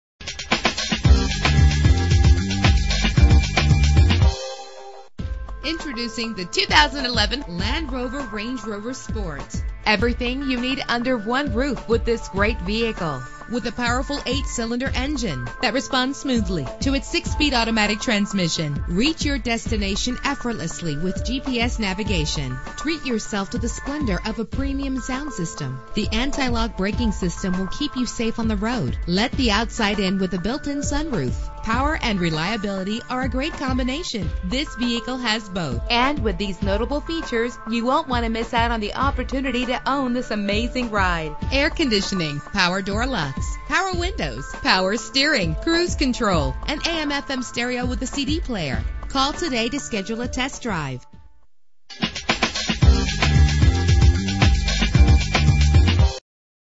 2011 Land Rover Range Rover Sport - Allentown PA
Year: 2011
Make: Land Rover
Model: Range Rover Sport
Trim: Supercharged
Engine: 5.0 liter 8 cylinder 32 valve
Transmission: 6-Speed Automatic
Color: Blue
Mileage:
Address:
5254 West Tilghman